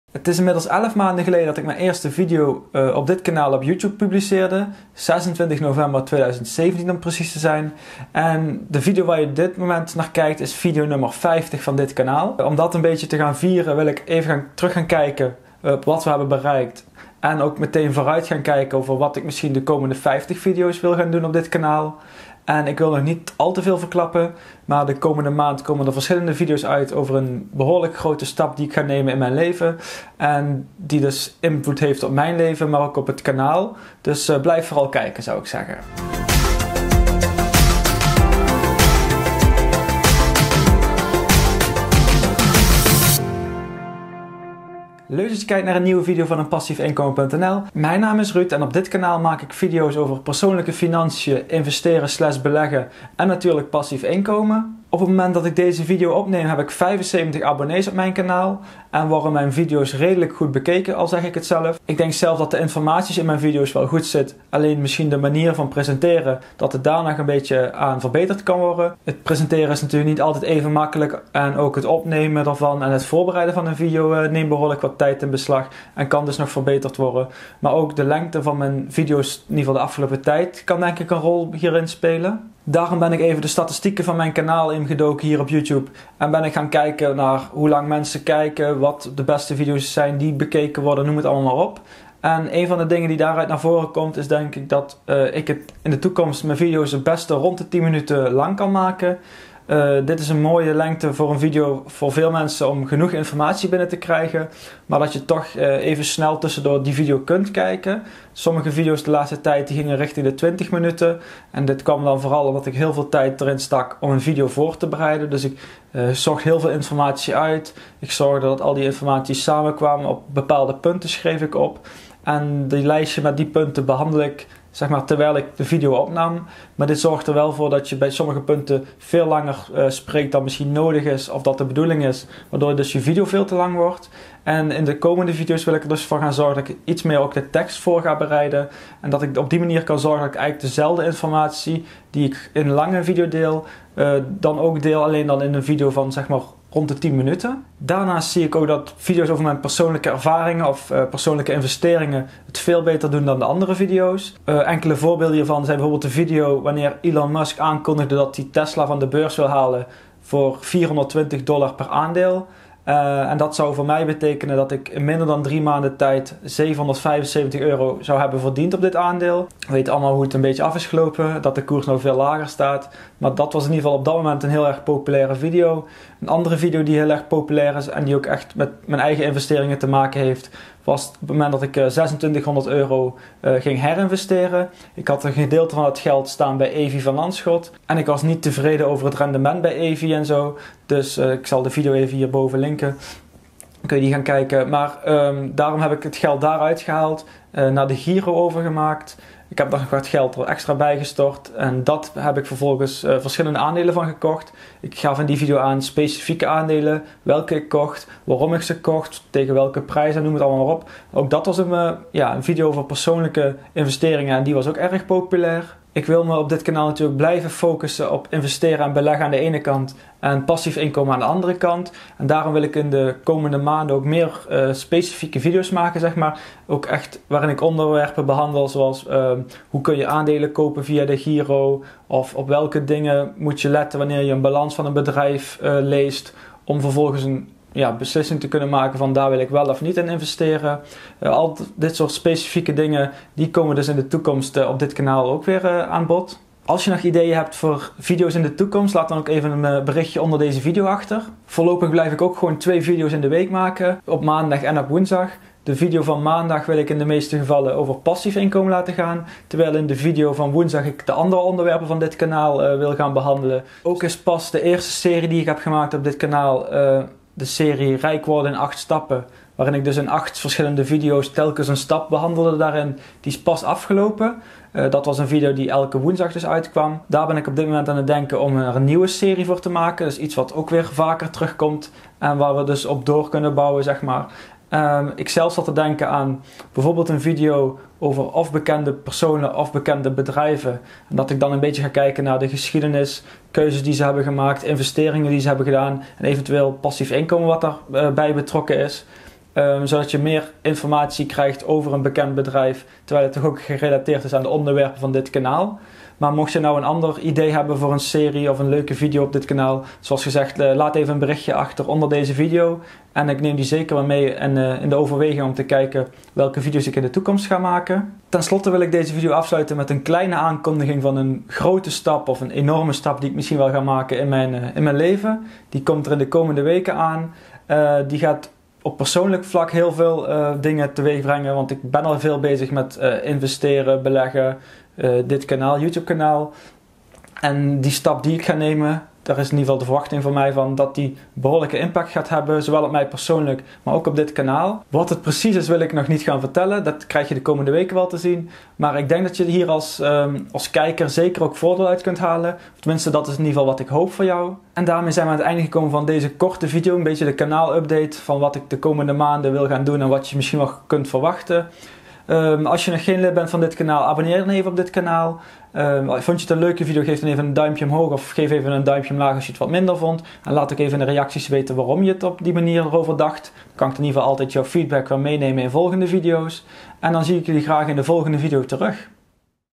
Kanaal update - Oktober 2018 | eenPassiefInkomen.nl
★★★ GRATIS DOWNLOAD ★★★
[ ook een aanrader als je geen student bent ;) ]

★★★ OVER DEZE VIDEO ★★★
In deze video, nummer 50 van dit kanaal, kijk ik een beetje naar het verleden maar vooral naar de toekomst en de volgende 50 video's. Wat zijn mijn plannen, wat kun je van me gaan verwachten de komende tijd en houd ik jullie nog iets langer in spanning over mijn grote stap die ik binnenkort neem.